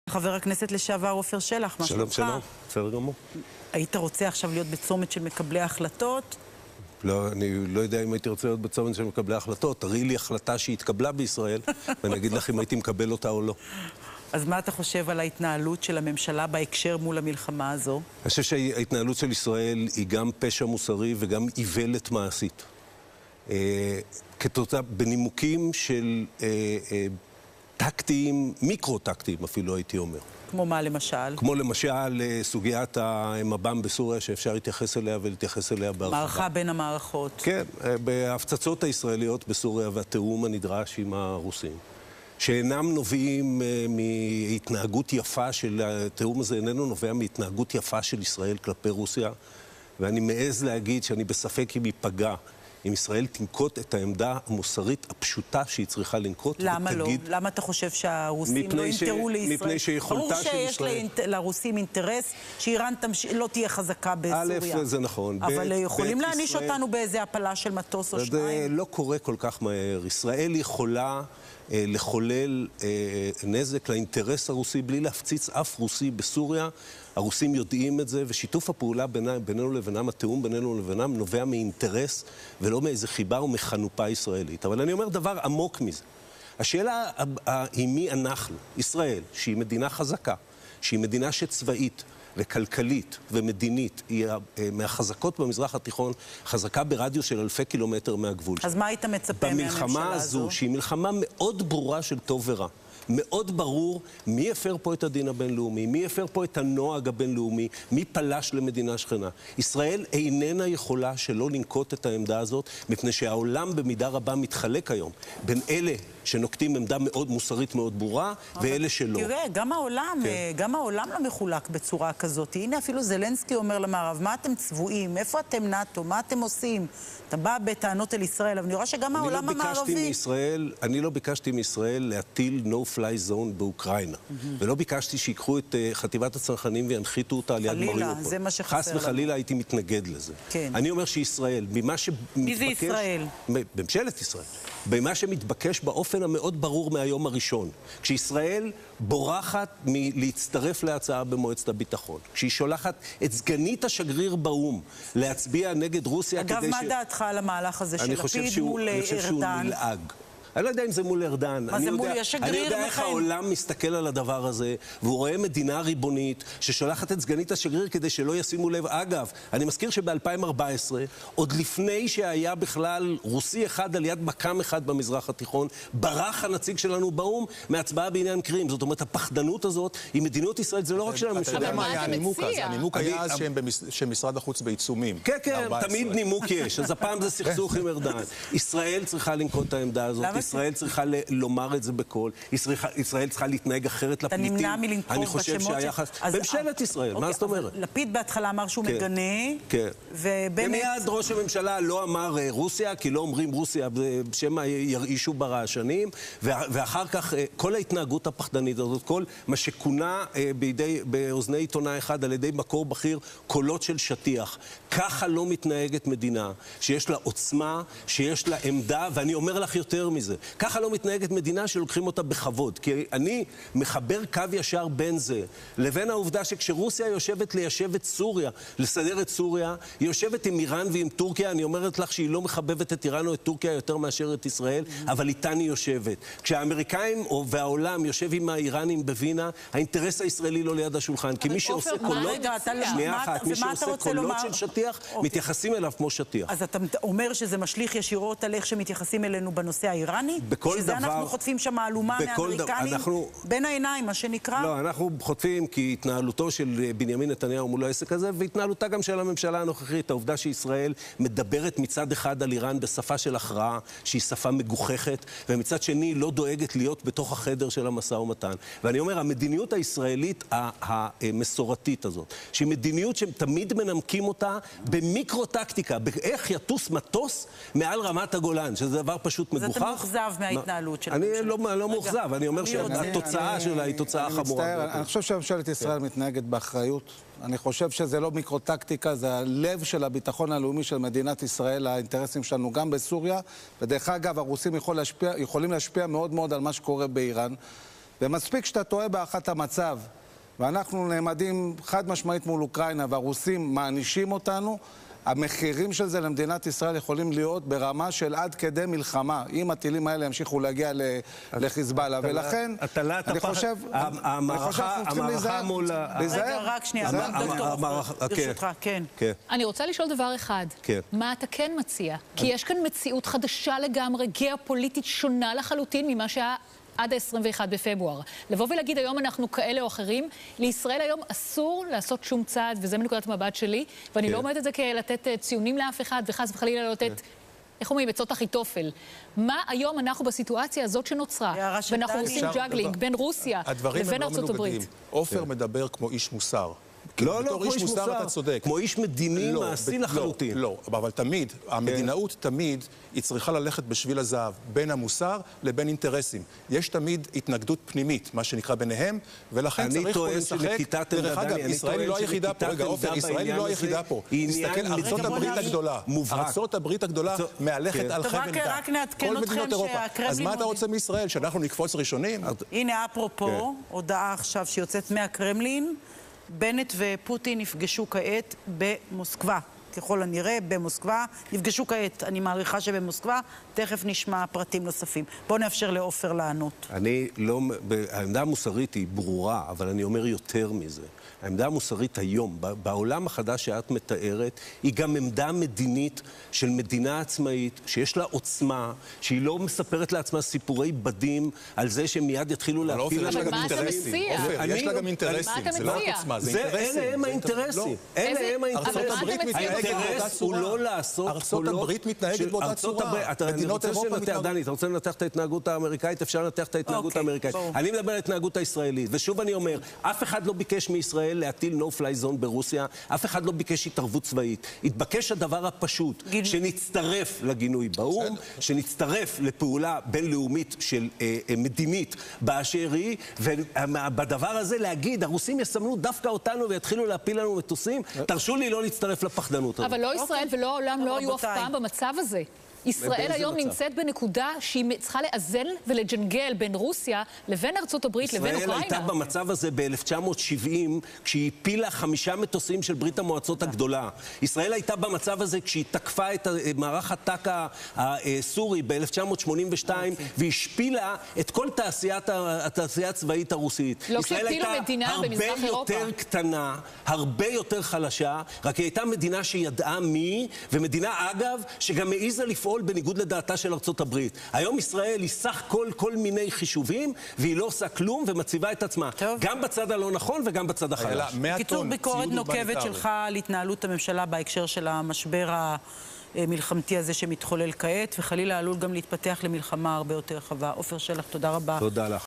את הנקמה מגישים כשהיא קרה ורצוי כשיריבך בנקודת שפל. עופר שלח מגיע לאולפן כדי לשלוח חיצים בחברו לשעבר
עופר שלח מגיע לאיילה חסון כדי לנקום בלפיד (כיכי אשם, אבל גם הממשלה הזו רופסת והתנהלותה היא פשע מוסרי ואיוולת מעשית).
קוטל את הממשלה ואת מהלכיה אבל מזכיר לנו מדוע זה טוב שהוא יצא מהממשלה.